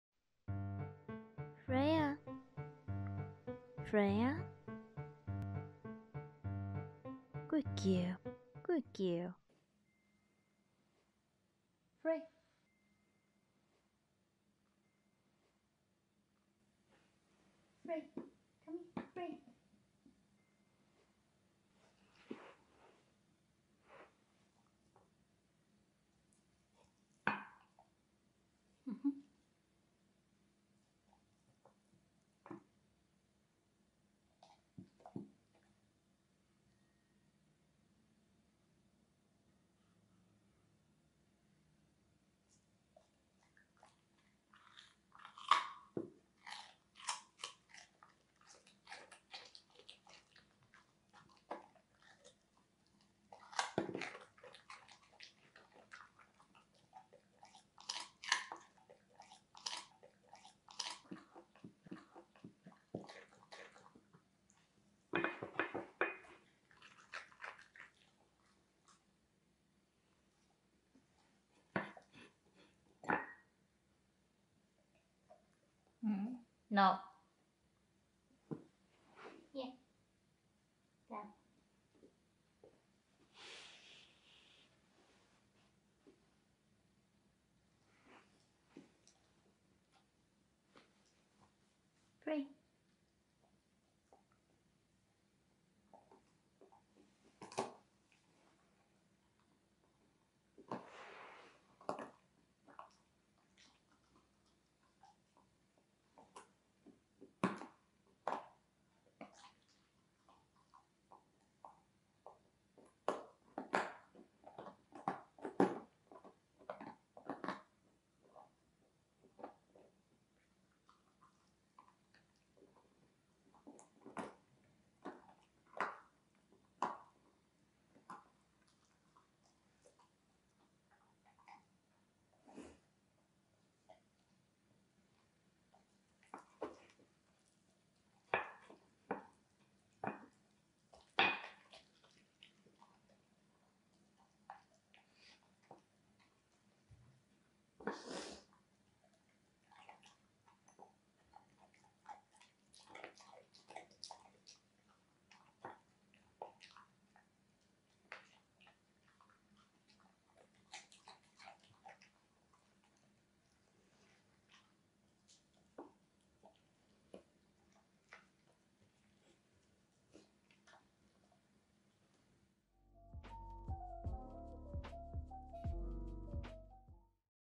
My dog prefers carrot to bone to salmon | ASMR | Dog reviewing food | Alaskan Malamute
It seems like Freya prefers carrot to pork bone to salmon